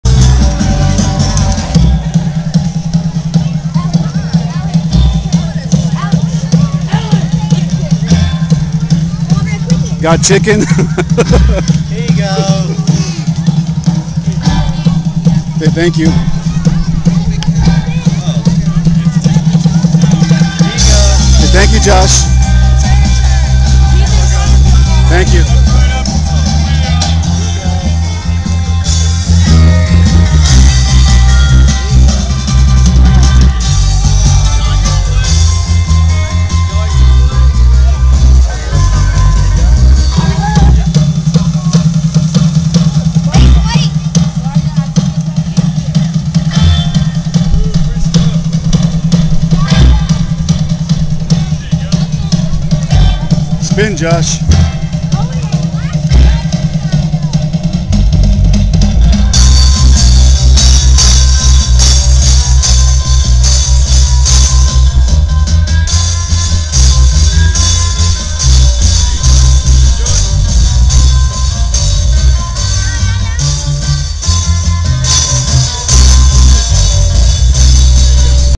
Tiki Blue & Gold Macaw Meets Chick-fil-A Cow Eat More Chicken
Tomball Texas Halloween Tiki Macaw Meets Chick-fil-A Cow Eat More Chicken Part 1
Tomball Historic Depot Plaza
Tomball has a Haunted Train Depot and Spooky Caboose and every year we enjoy coming down from the Woodlands